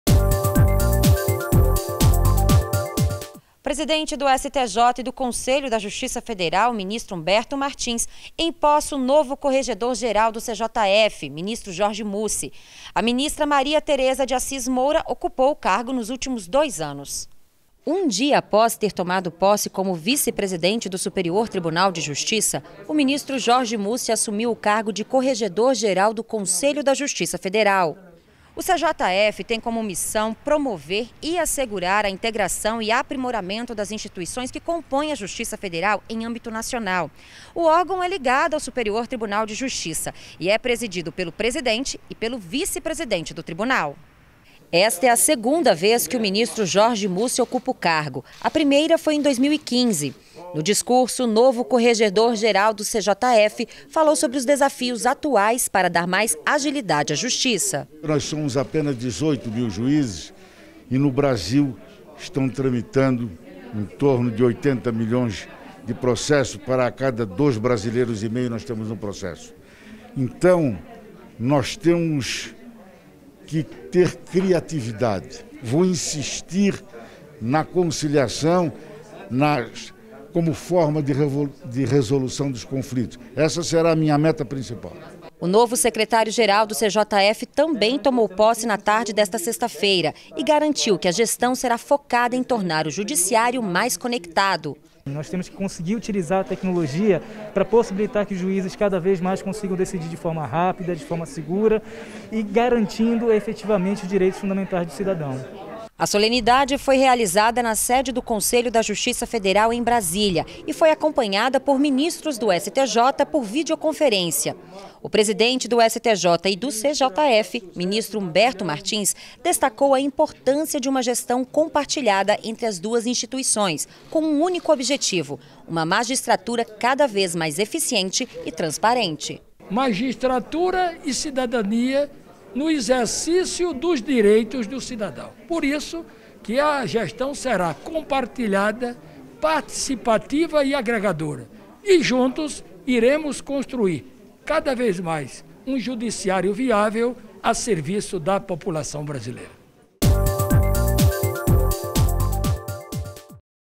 Ministro Jorge Mussi toma posse como corregedor-geral da Justiça Federal
​​​​O vice-presidente do Superior Tribunal de Justiça (STJ), ministro Jorge Mussi, foi empossado na última sexta-feira (28) no cargo de corregedor-geral da Justiça Federal. O ministro disse que vai dar ênfase à conciliação na tentativa de fazer com que a prestação jurisdicional ocorra em tempo razoável.